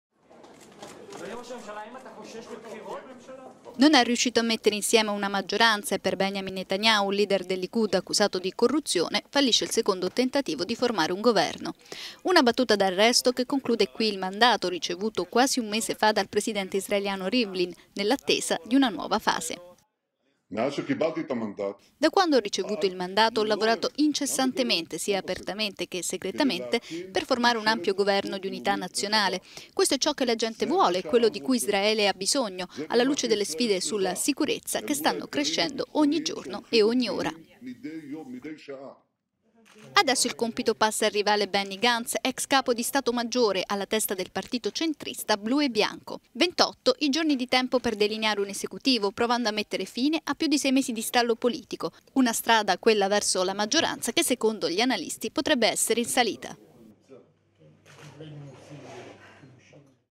Israele: Netanyahu rinuncia a formare il governo
Israele: Netanyahu passa la mano. Tocca ora a Benny Gatz provare a trovare una maggioranza alla Knesset.…